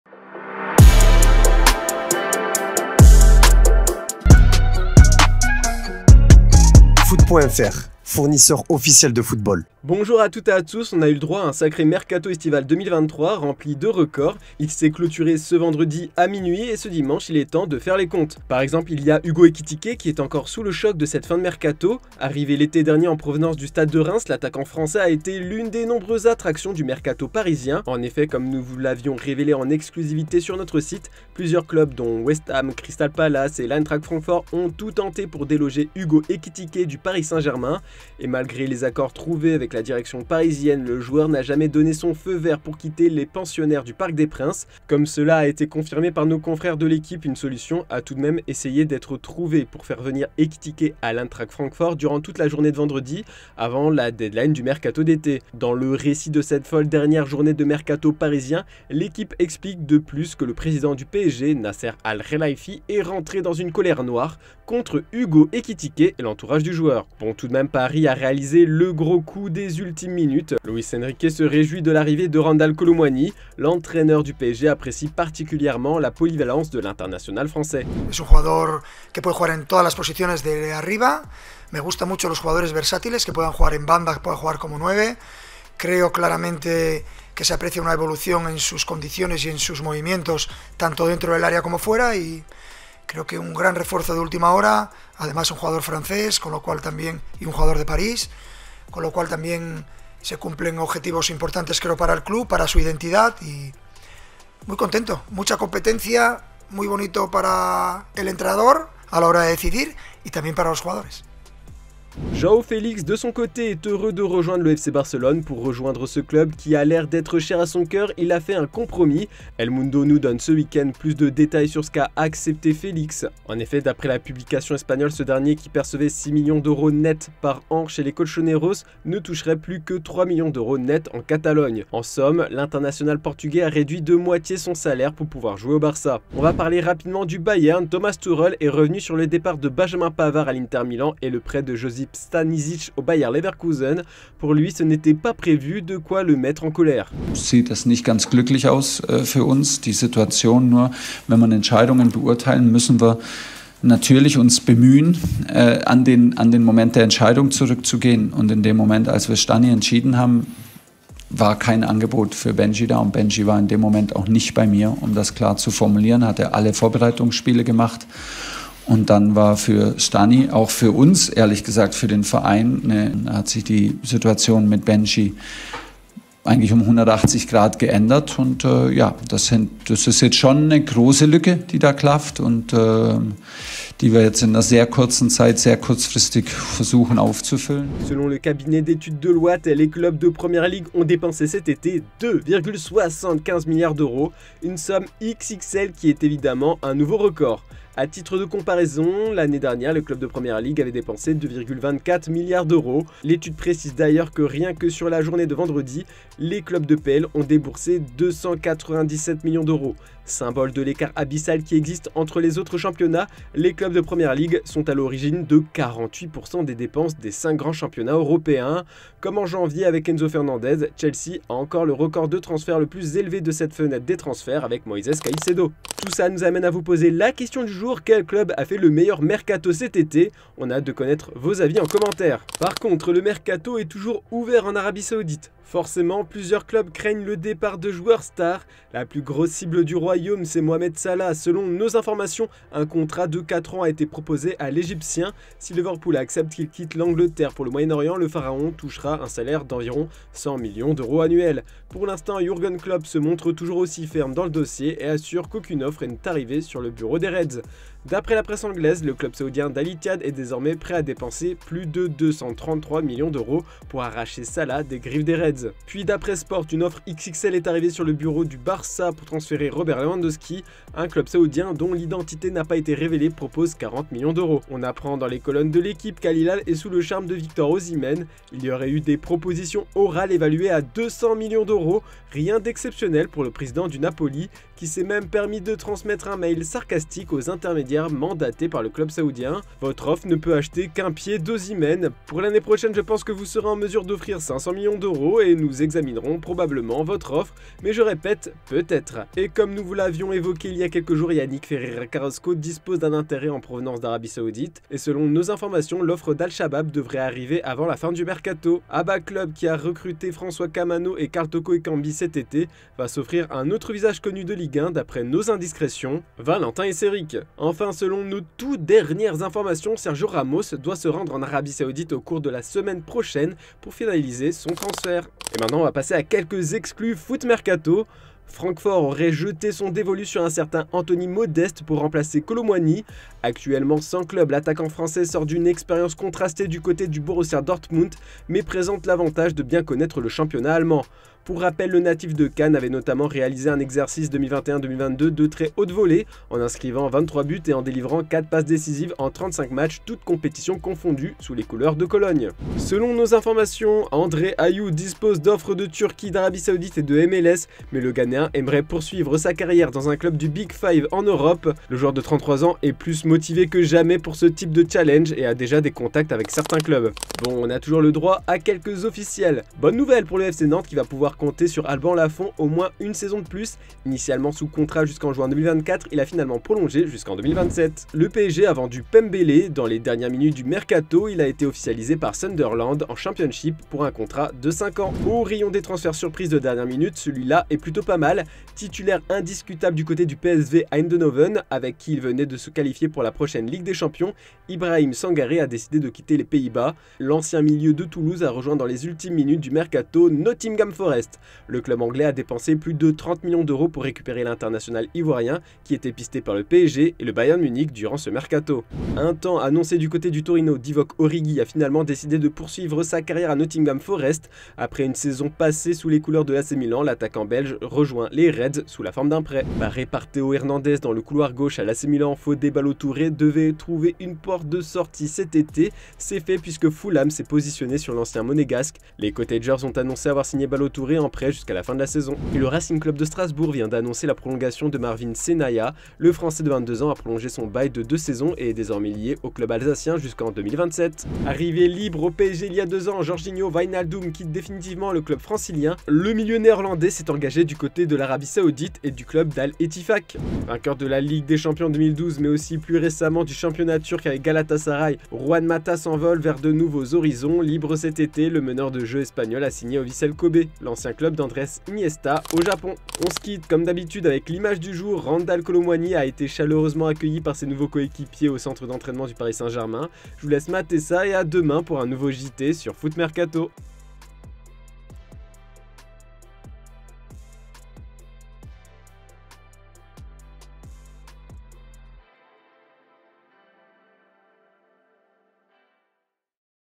Les OFFRES INDÉCENTES de l'Arabie saoudite pour 3 STARS du football mondial | JT Foot Mercato
Au programme de ce JT Foot Mercato : le championnat saoudien veut encore de renforcer avec quelques stars, Joao Felix a fait un cadeau au FC Barcelone et les officiels du jour.

00:00 Introduction
00:09 Ekitike dans la tourmente
01:52 Le sacrifice de Joao Felix
03:45 La question du jour
03:51 Le mercato fou de l'AS
05:25 Les exclus du jour
06:07 Les officiels du jour
07:21 L'image du jour